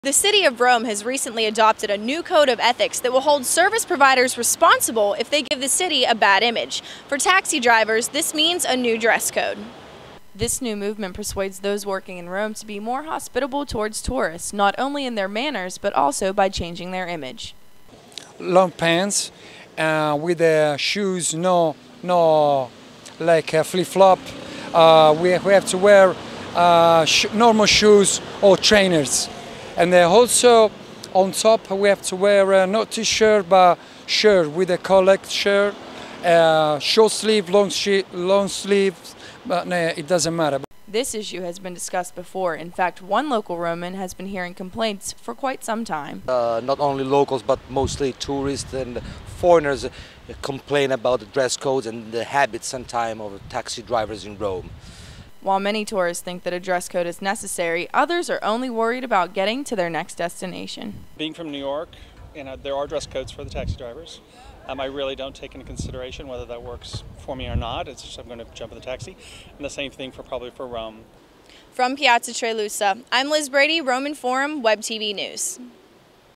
Taxi Drivers Dress for Success
Rome, Italy, July 26, 2009 - The City of Rome has recently adopted a new code of ethics that will hold services providers responsible for giving the city a bad image - for taxi drivers this means dressing smartly.
Taxis, Rome, taxi-drivers, ethics, dress code, tourists